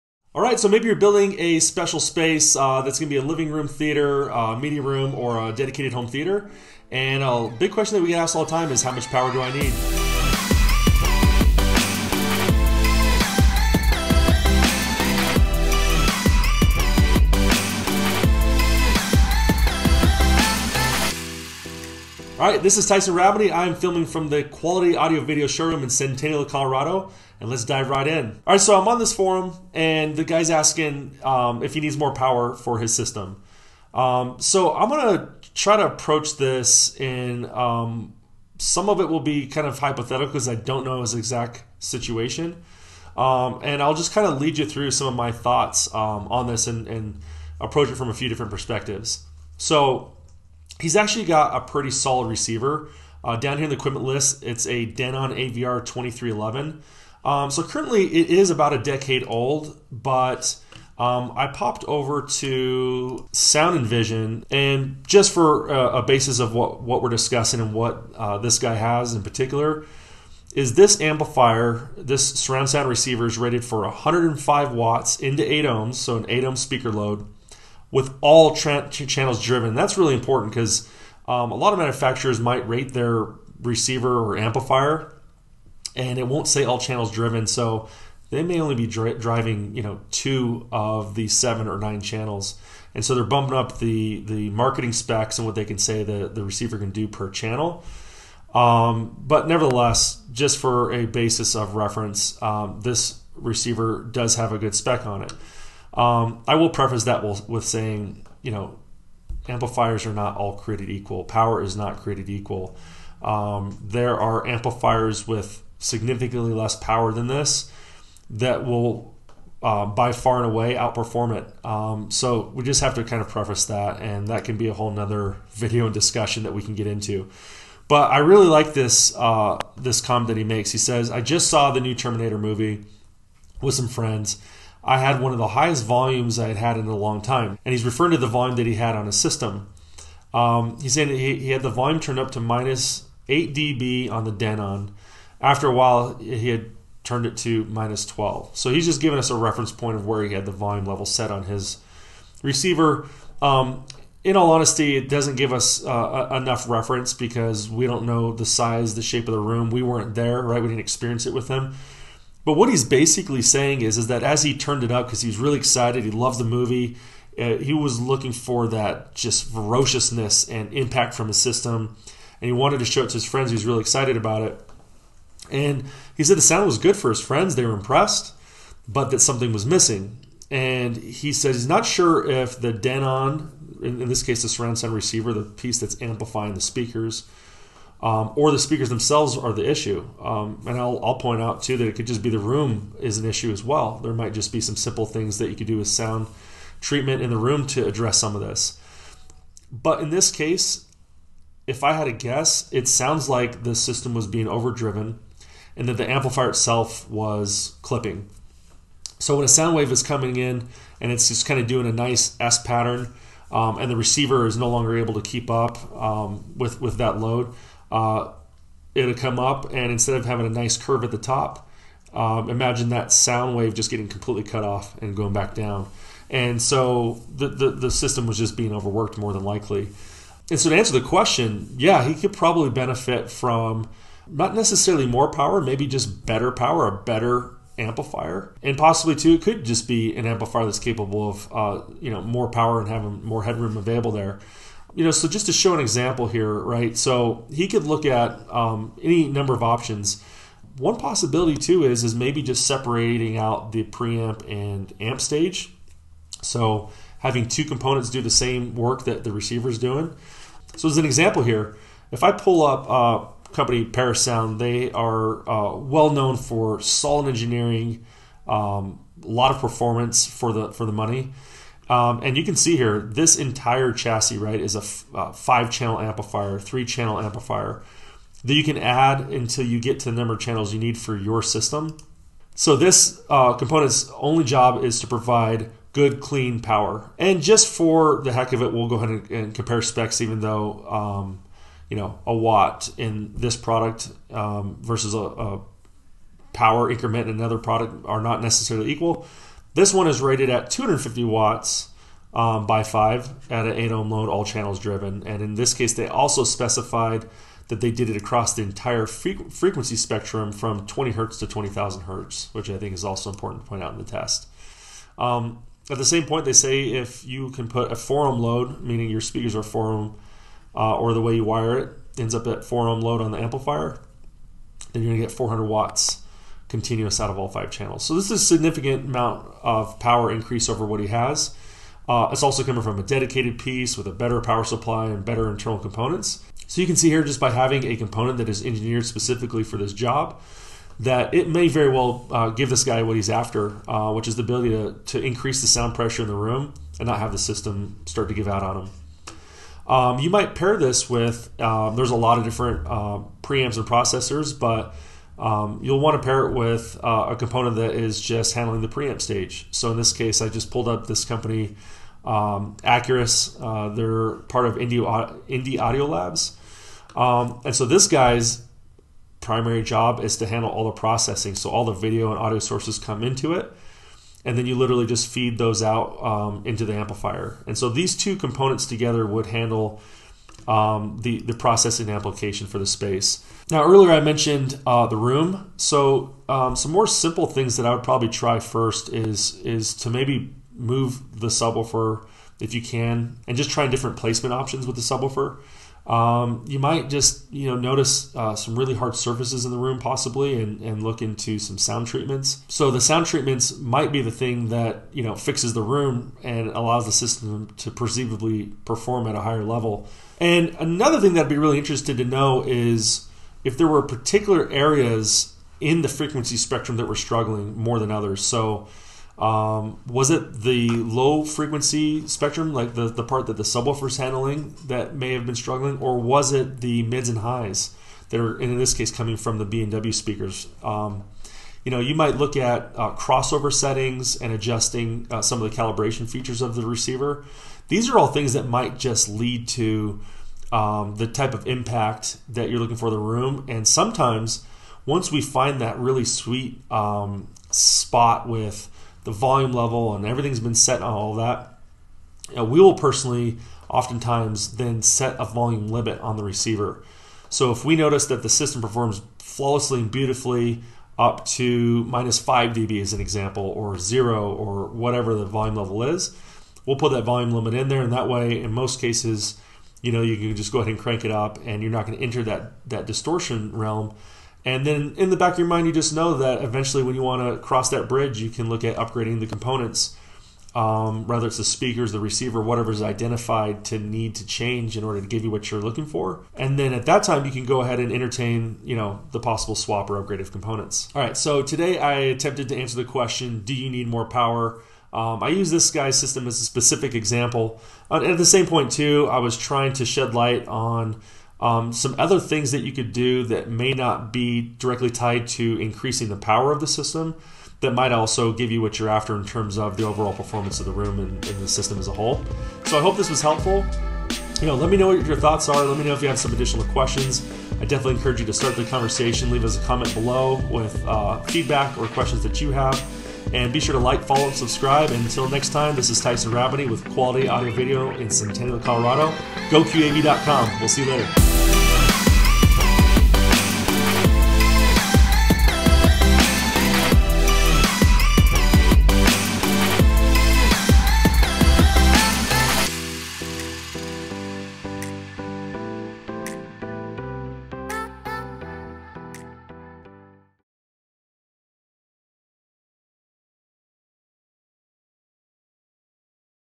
Do I Need a More Powerful Receiver for my Home Theater?
One of the biggest questions we get asked at GOQAV in our Denver based custom audio store is, "Do I Need a More Powerful Receiver for my Home Theater?"

There are many factors that tie into this question and so we take a question from and AV forum and dive into a specific scenario where this person is using a pretty solid receiver, the Denon AVR-2311 and although it's about 10 years old we will dive into the specs and ways to make this amplifier perform the best in the room.

00:30 - Do I need more power for my amplifier and system ...

1:04 - Specs of amplifier and ratings ...

1:50 - All amplifiers are not created equal.  Power is not created equal.

2:00 - There are amplifiers on the market with significantly less power than this that will out perform it but we will get into that in another video.

2:20 - Scenario for what was happening in the room.

2:45 - What happens when the volume is cranked?  Is the amplifier failing or speakers not performing?

3:25 - S patterns and clipping.

3:55 - Perhaps it's not about more power, but better power.

4:14 - Separate out the preamp and the amp stage as on option.

4:33 - Parasound Halo Amp A51 Specs

5:50 - What to pair the Halo Amp A51 with a Media Processor

6:26 - Other simple scenarios to help improve the performance in your room including sound treatments or subwoofer placement.

7:08 - Was it the lows, or mids and highs that were having issues?  Check cross over settings and adjusting the calibration settings on your receiver.

7:36 - Setting a volume limit on the receiver.

8:05 - outro

MB01HH7UIEPDXB4